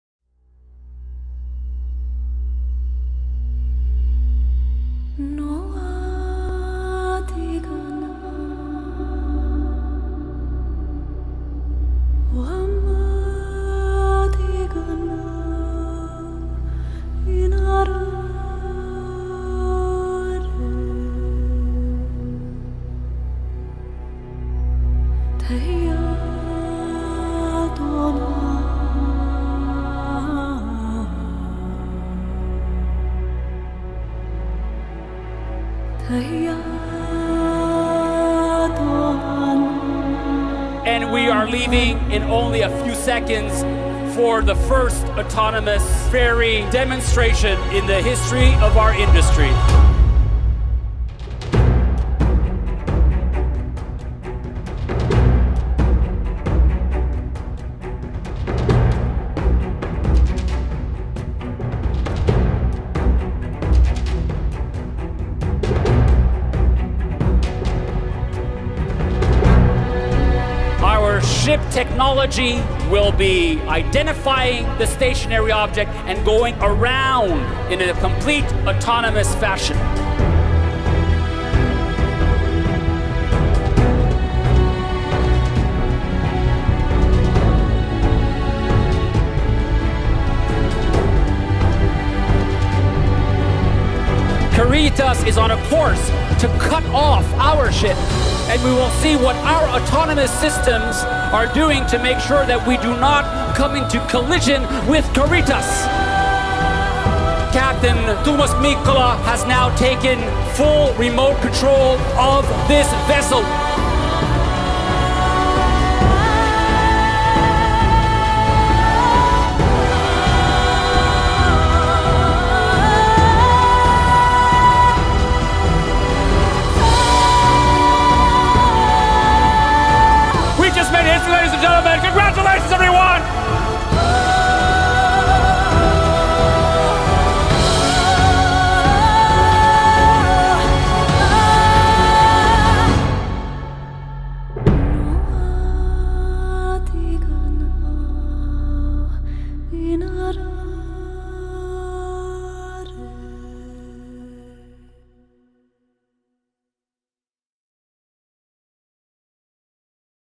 Rolls-Royce İlk Otonom Gemisini Test etti
Rolls-Royce, önümüzdeki 6 yıl içerisinde prototip otonom kargo gemileri olan ilk otonom gemiyi test etti.